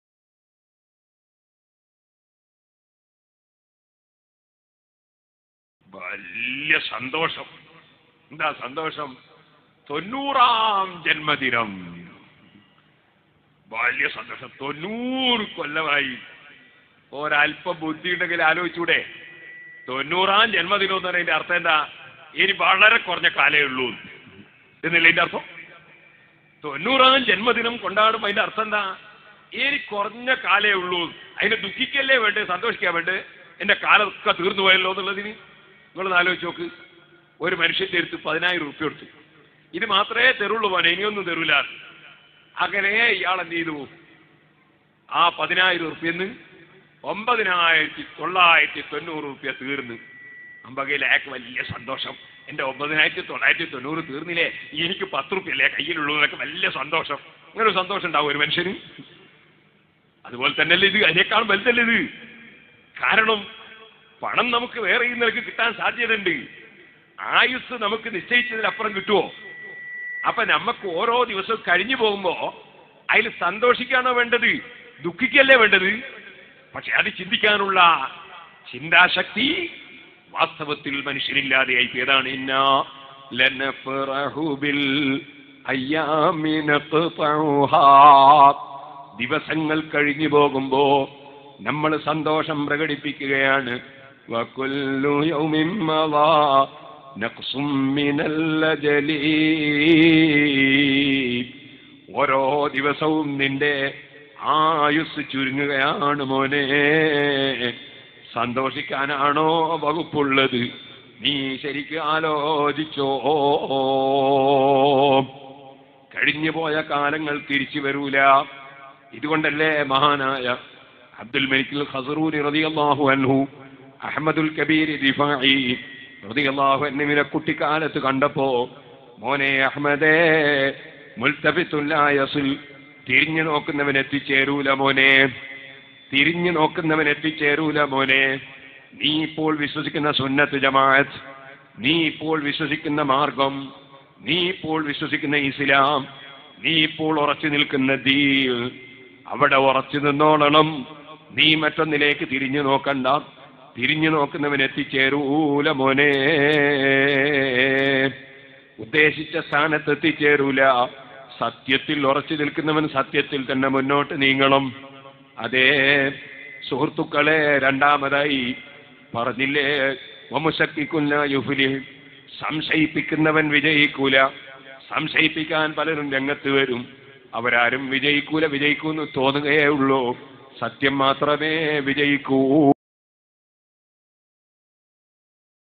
കേട്ടിരുന്നു പോകും ഈ പ്രഭാഷണം | Perod Usthad | Islamic Speech
നിങ്ങൾ BIRTHDAY ആഘോഷിക്കുന്നവരാണോ...

ഈ വീഡിയോ നിർബന്ധമായും കാണുക.